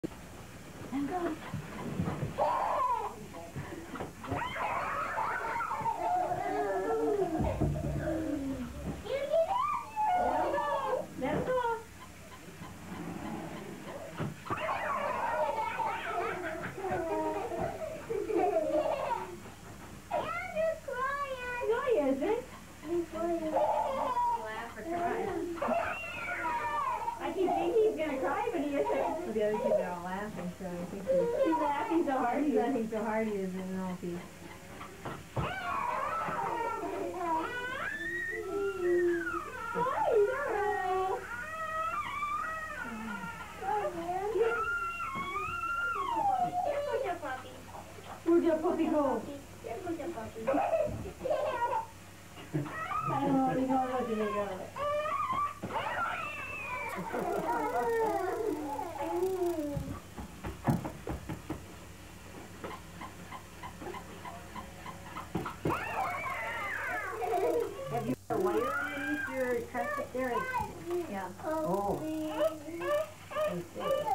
Andys Birthday Toy
Andy receives a new flipping dog toy for his first birthday, but we are not sure if he likes it or not. He laughs one moment and cries the next. His brother Shaun really likes the toy.
This was filmed in 1990.